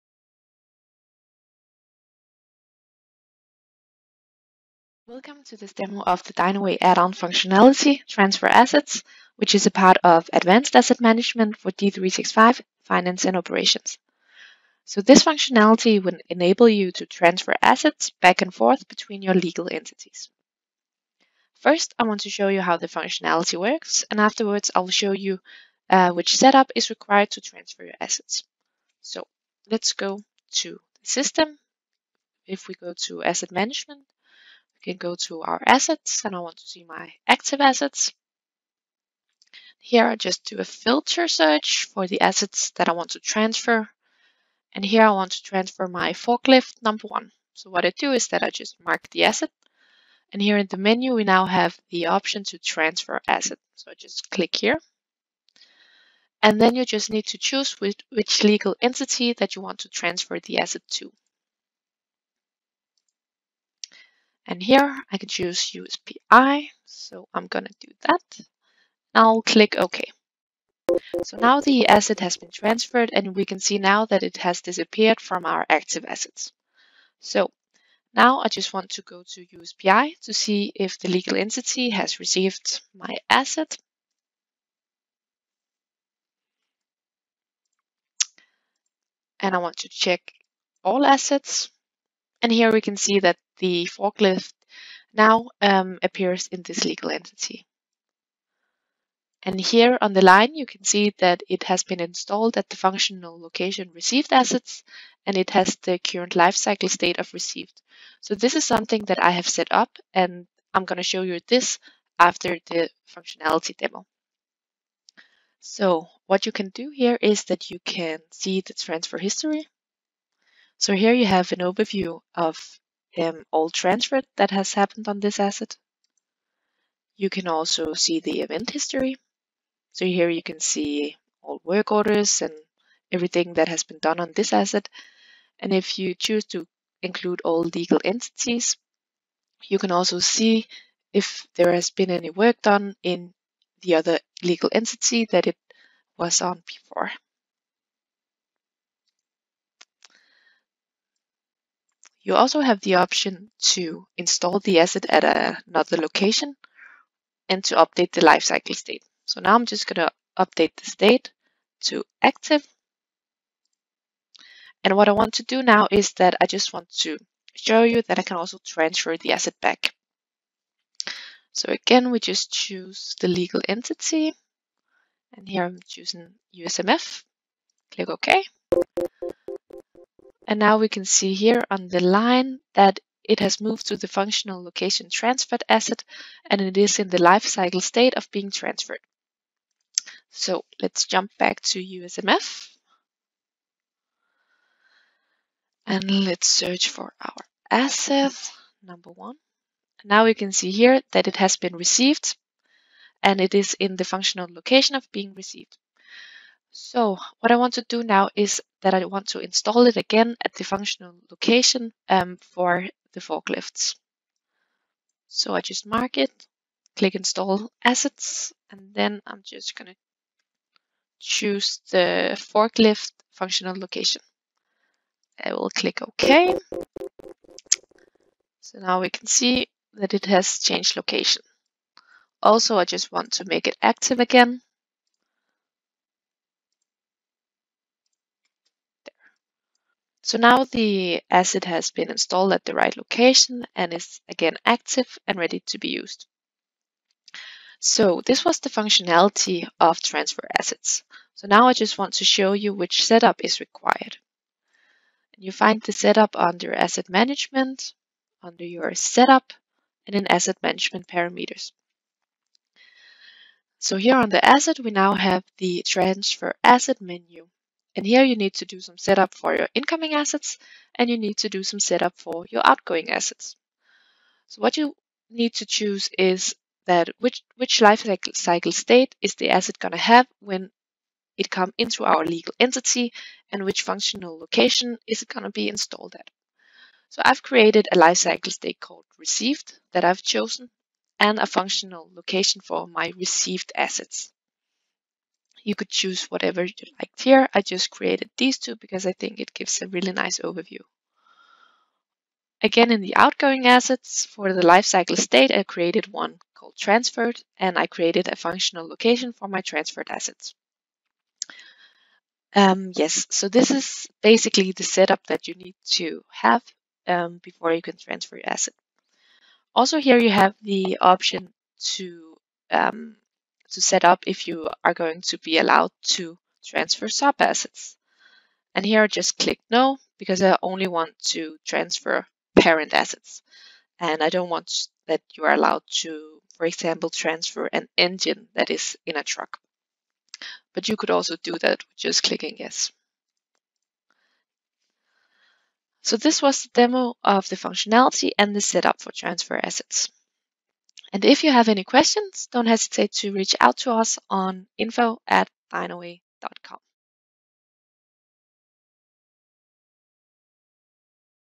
Microsoft Asset Management: Transfer your Assets to a different legal entity
The main functionality of transfer assets is that you can transfer selected assets and sub-assets to a different legal entity and receive assets from a different legal entity.

Dynaway
Enterprise Asset Management solutions
Alfred Nobels Vej 21
Aalborg 9220